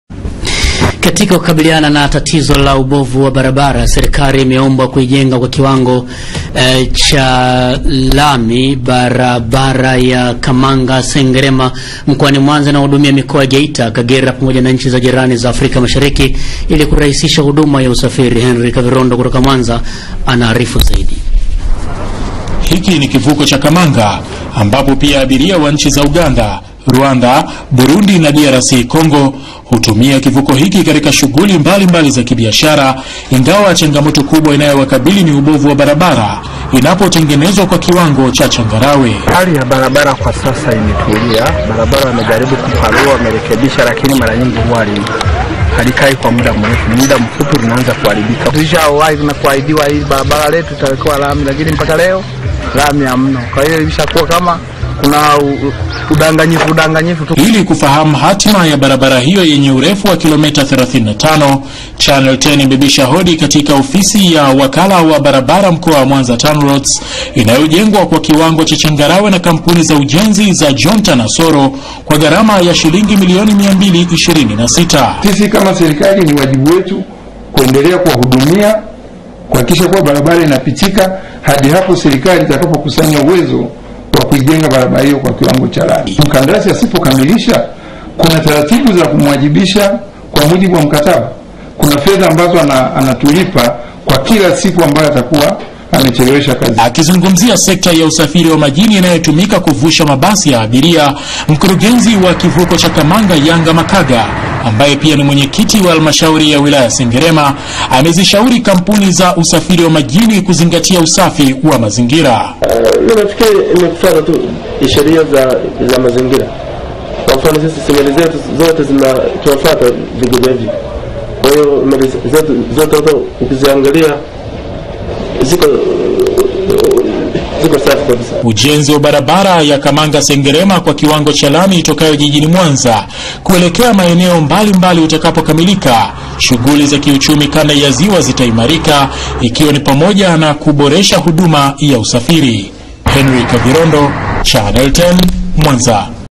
Sengerema Walia Na Ubovu Wa Barabara
Serikali imeombwa kuijenga kwa kiwango cha lami barabara ya Kamanga hadi Sengerema mkoani Mwanza ili kuwaondolea wananchi adha ya usafiri kipindi cha mvua.